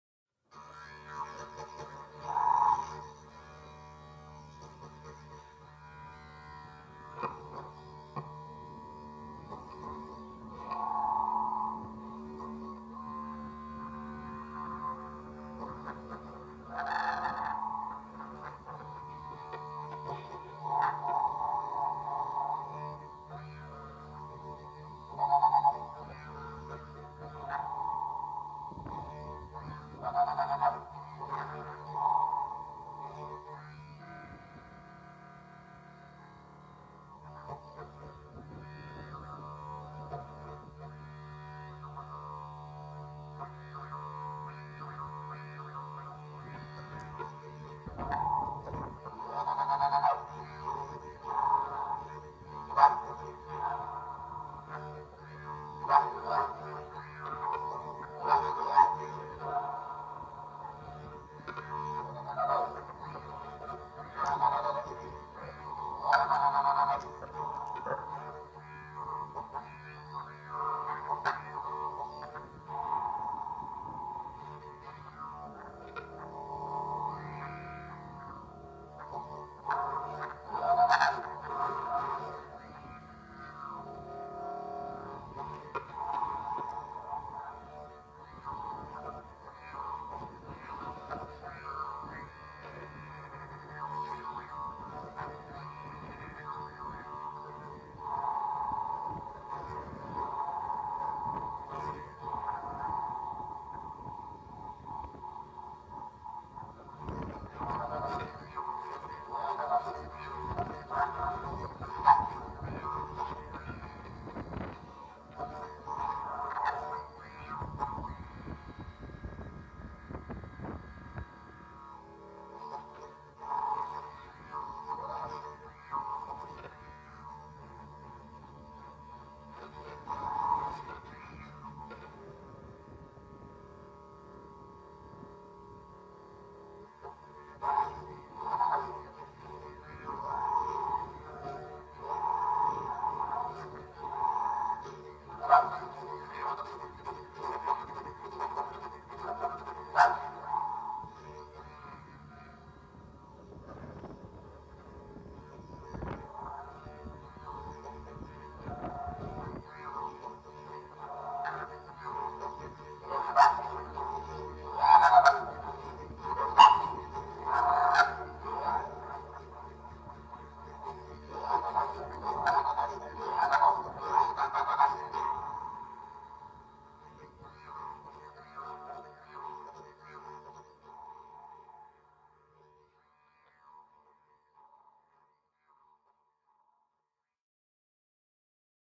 Mad Composer Lab - Silverwood Lake Meditation Session 2
In the forest there's a giant boulder near Silverwood Lake.

Great space for playing yidaki and witnessing nature.